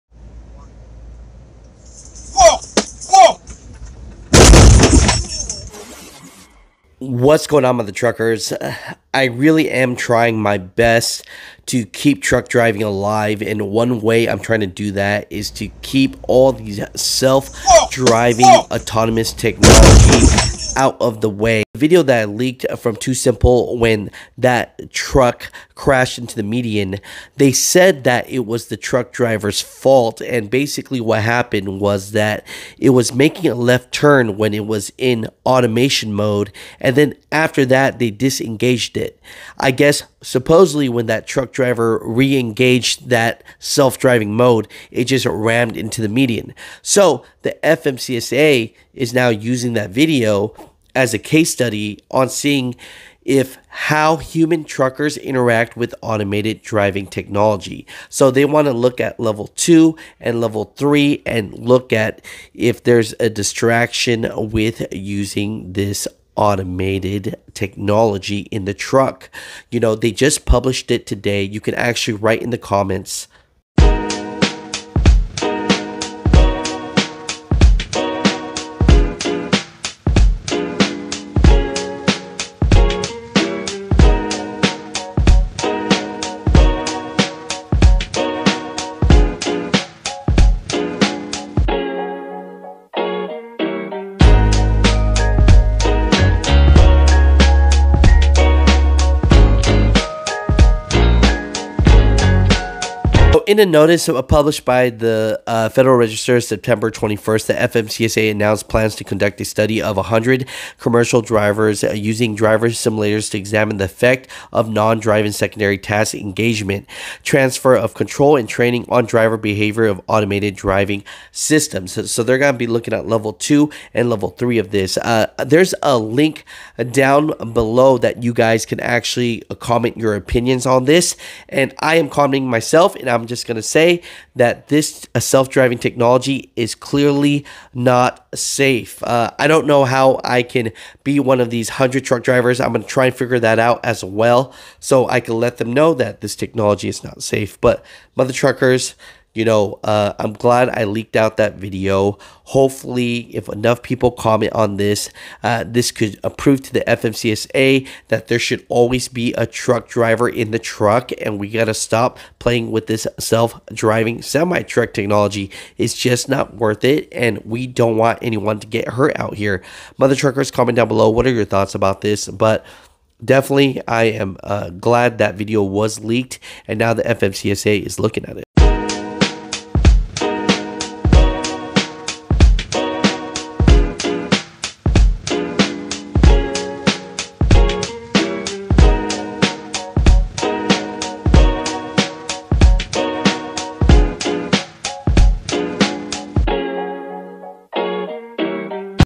3.3 Million Truck Drivers Jobs On The Line! FMCSA Reaching Out To All Truck Drivers 😵
Just Announced! FMCSA Is Asking 3.3 Million Truck Drivers About Self Driving Semi Trucks